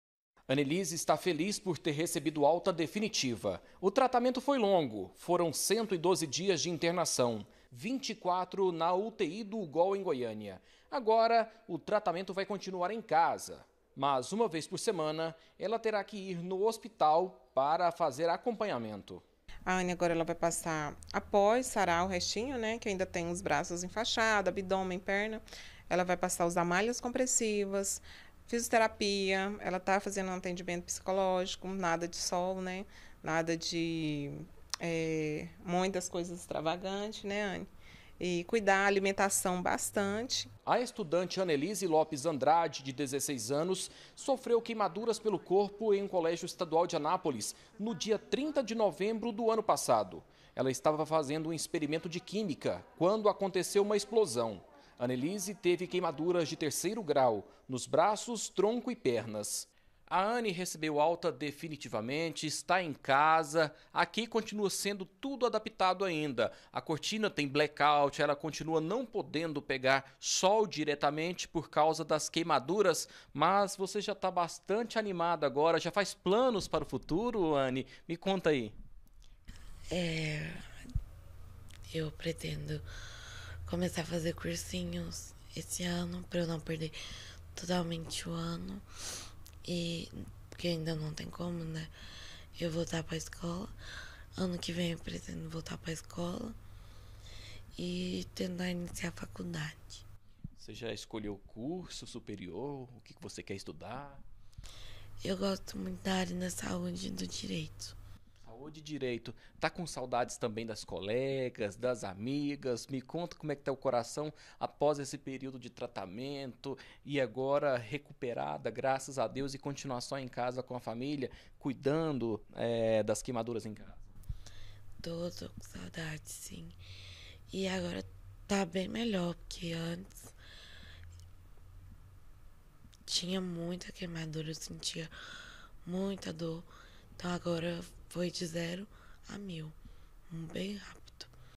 ALTA HOSPITALAR: ESTUDANTE QUEIMADA VOLTA PRA CASA
DEPOIS DE PRATICAMENTE QUATRO MESES DE INTERNAÇÃO, FINALMENTE TEVE ALTA A ESTUDANTE DE ANÁPOLIS QUE SOFREU QUEIMADURAS POR TODO O CORPO DURANTE UM EXPERIMENTO DE QUÍMICA NA ESCOLA, NO FIM DO ANO PASSADO.